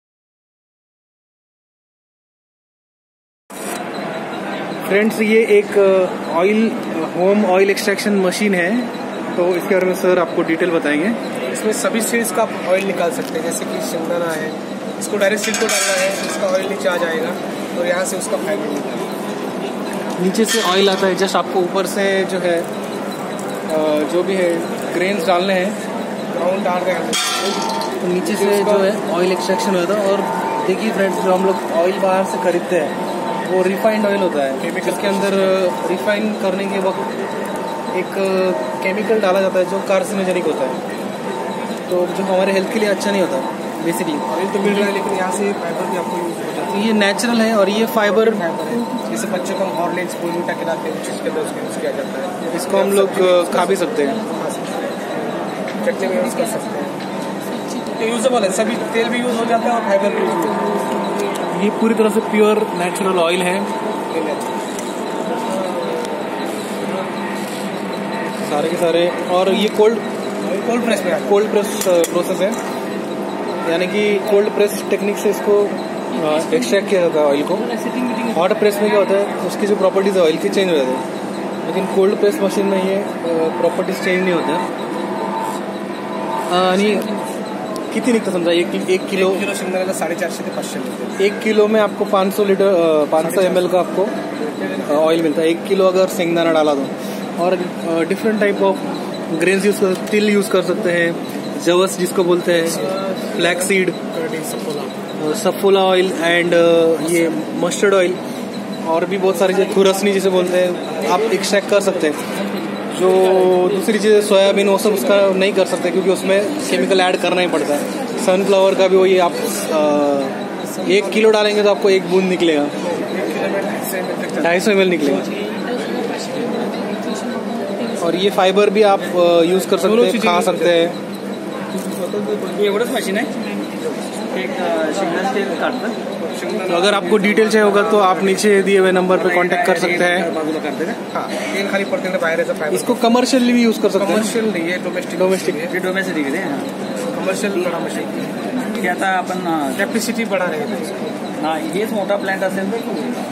घर में तेल निकले , केमिकल फ्री और नेचुरल तेल खाए
ghar pe oil extract karne ka machine,  bahar ke
रिफाइंड तेल में हार्मफुल केमीकल से बचे...